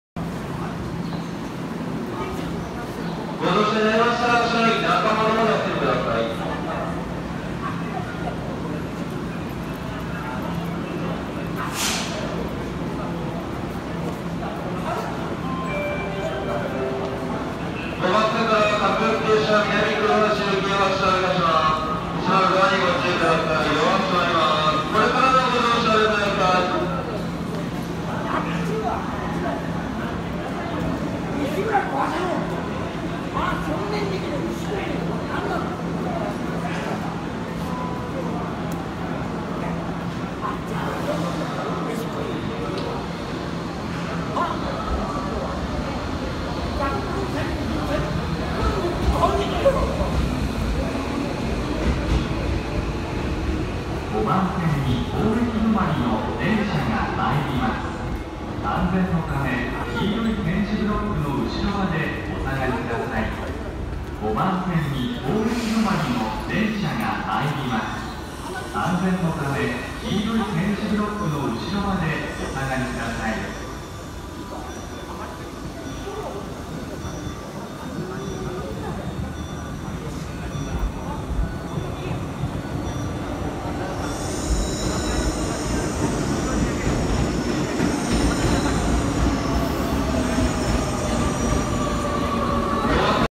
駅で鉄道警察ともめるオッチャン
駅で一触即発